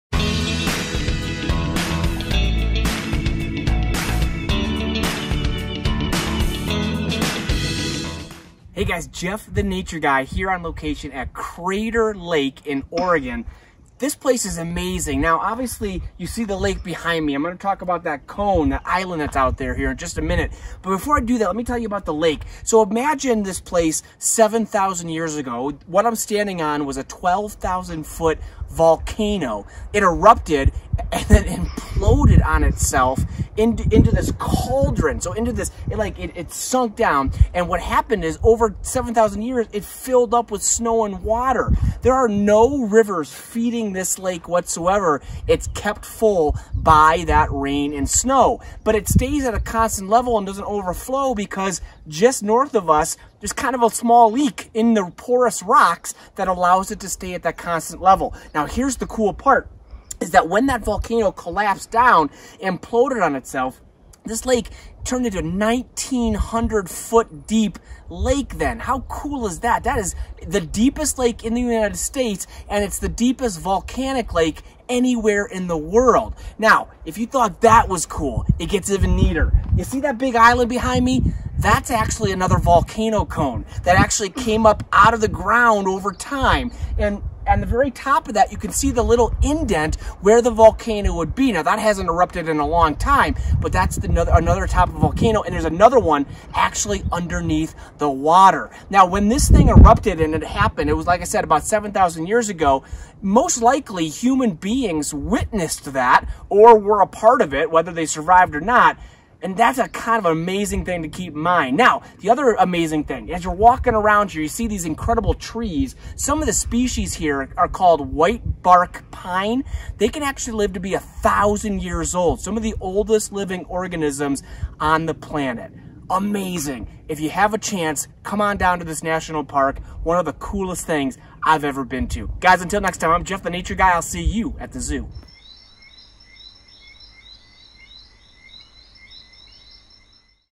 Exploring Crater Lake National Park
Jeff the Nature Guy explores Crater Lake National Park in Oregon, and talks about how this amazing volcanic lake was formed.